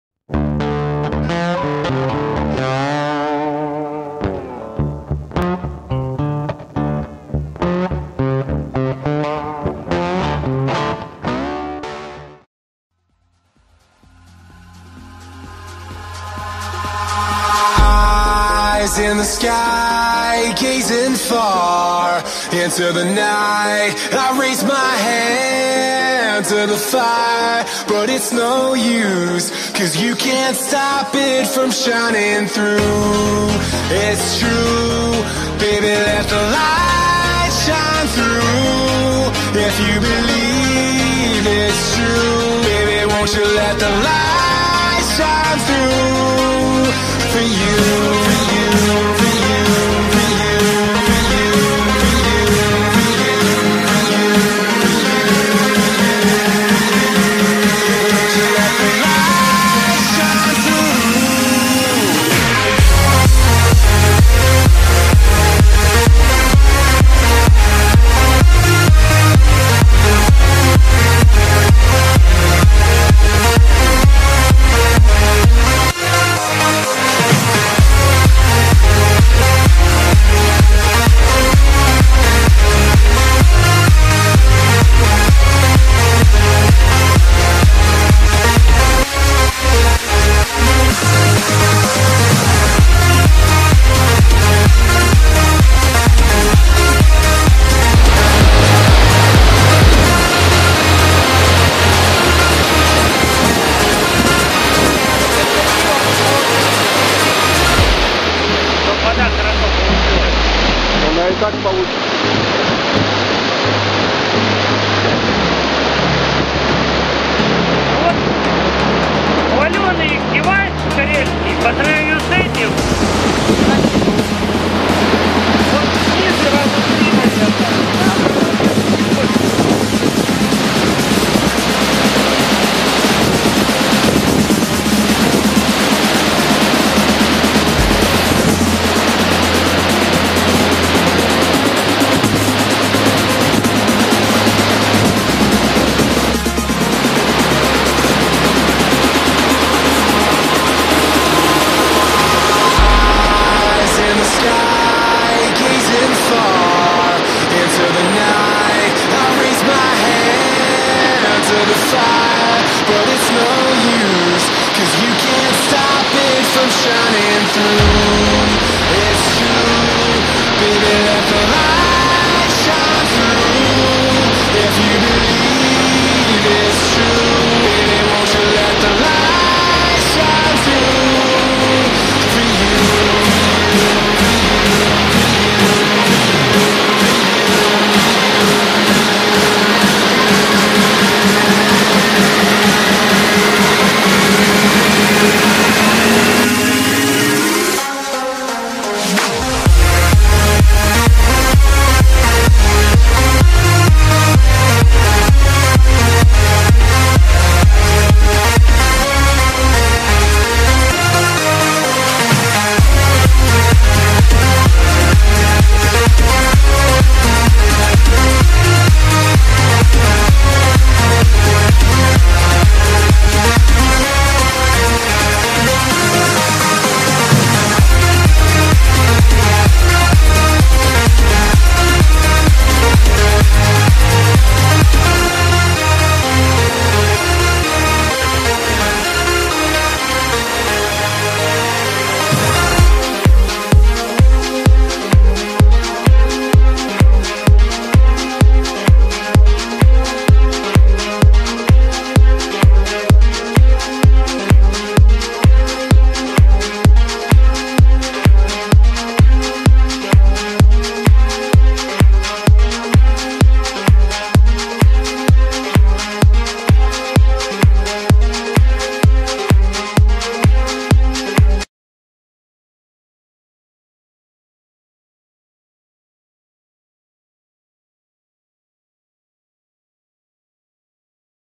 Водопад Эбручор. Лапландский заповедник.
Поход на водопад Эбручор.  Мончетундра. Лапландский заповедник.